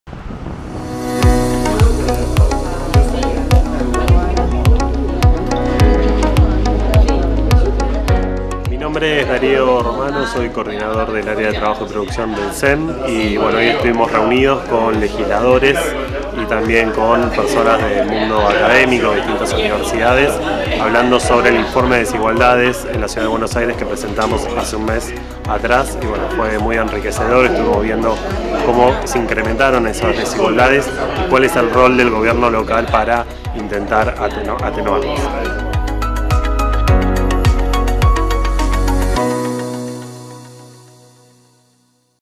Encuentro entre diputados y legisladores para conversar sobre desigualdades en CABA
Encuentro entre diputados, diputadas y legisladores para escuchar los resultados del último informe de desigualdades de la Ciudad de Buenos Aires que hizo el CEM en la UMET